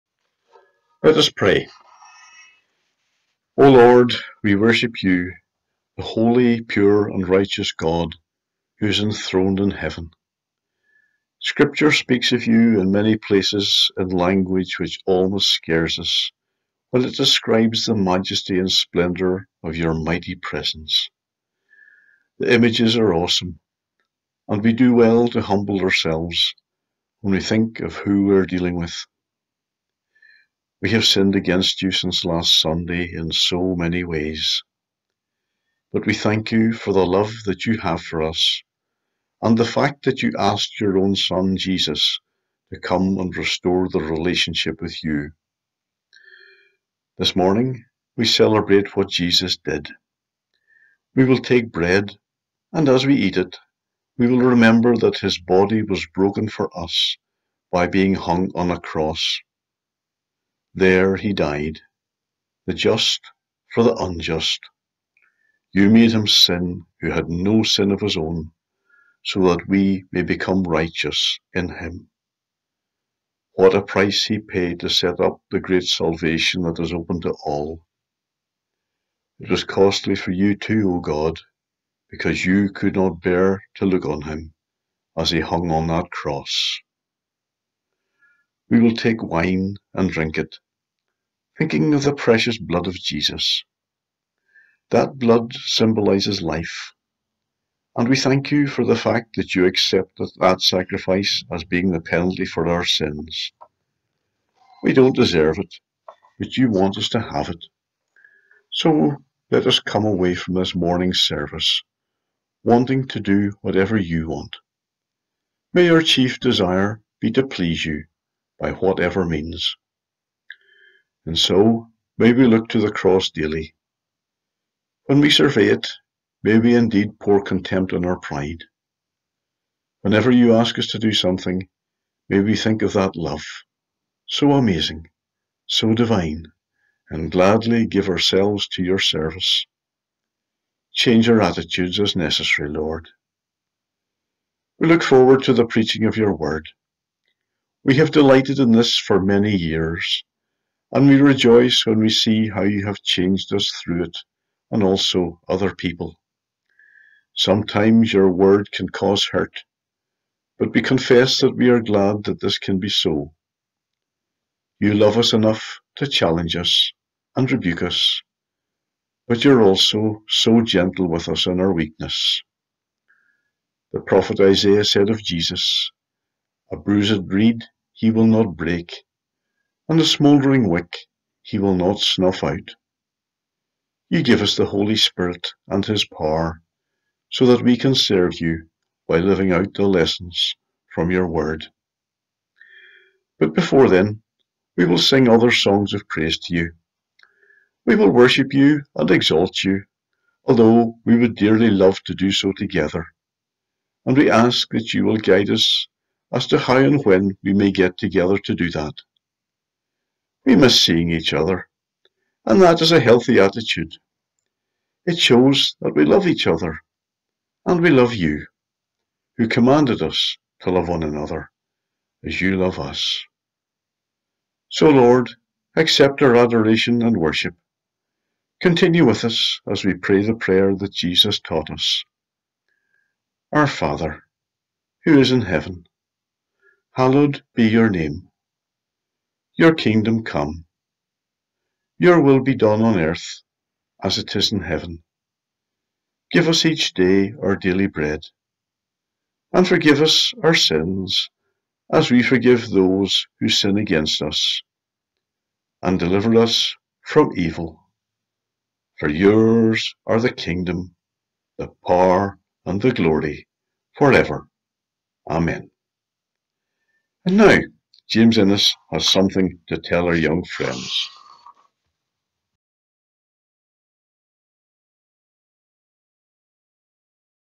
Opening Prayer - Sunday 4 October 2020 - Morning Service
Opening Prayer for the 11 am online Morning Service on 4 October 2020, from Sandyford Henderson Memorial Church.